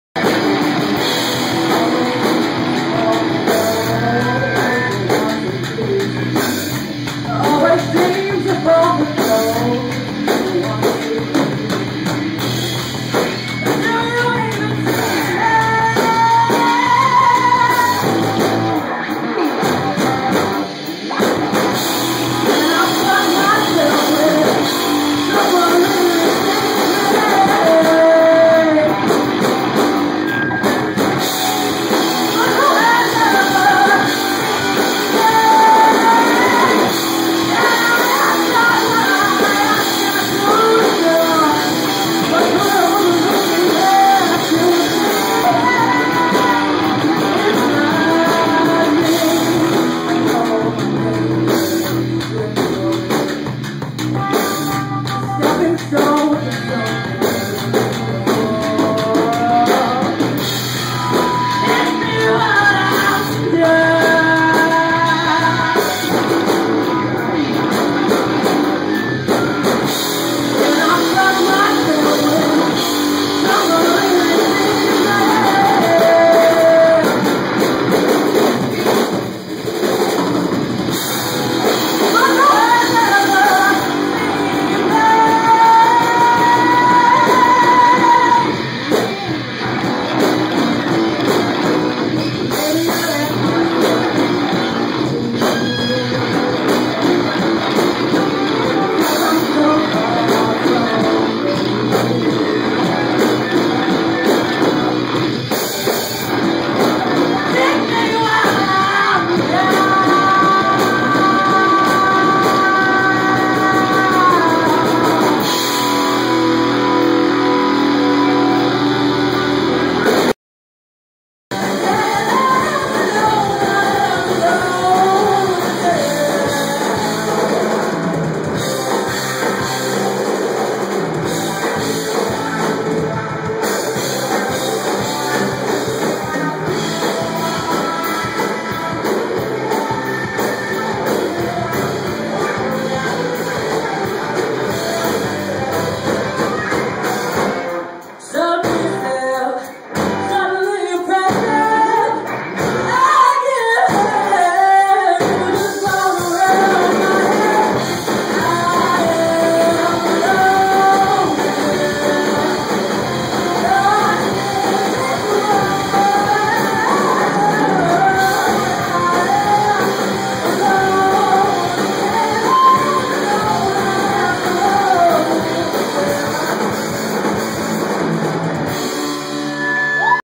Dr Hunter Live July 2013
Camera video of Dr Hunter playing in Philadelphia.